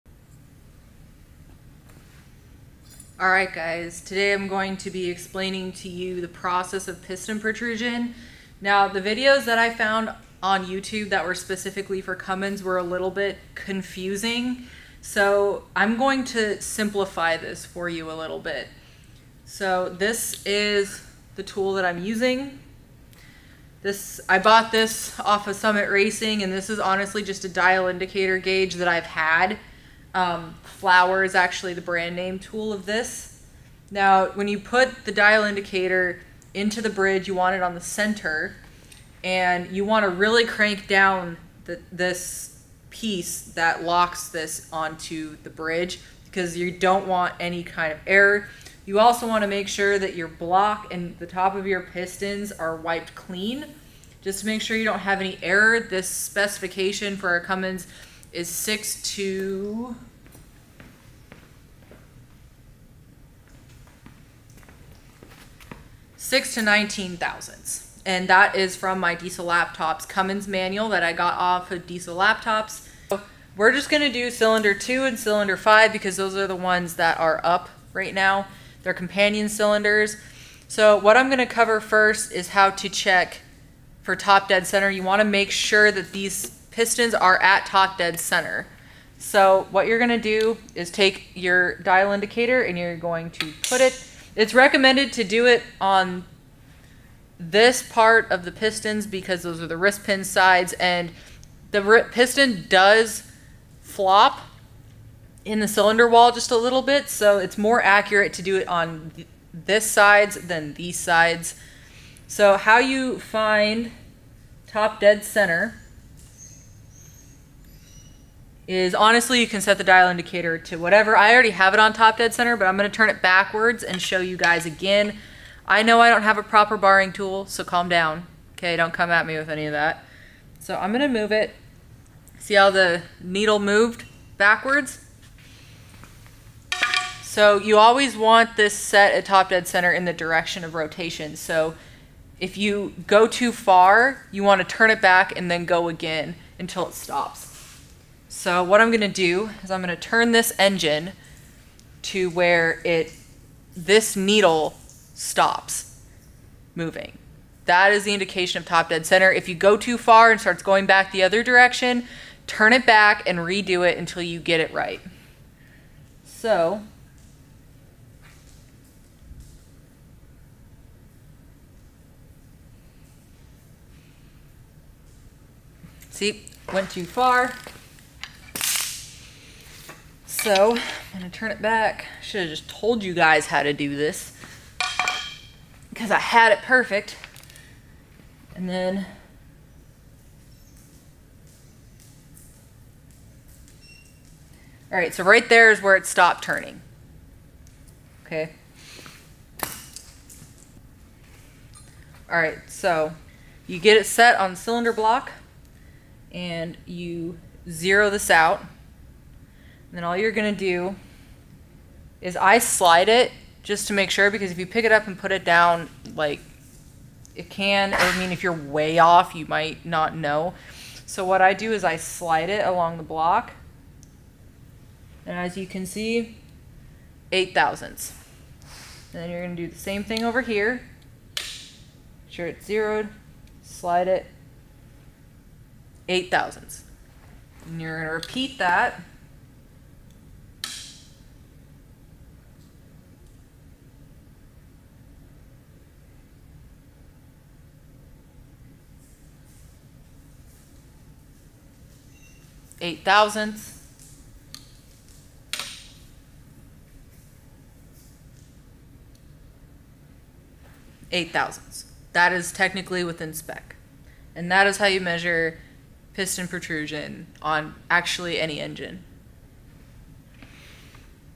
How To Measure Piston Protrusion
This is for a 5.9 cummins isb, but this procedure works for any engine.  Just make sure you look up the proper specs for your specific engine.
@DieselLaptops can provide you with a program called Diesel Repair. They have full manuals for almost every diesel engine in almost every application.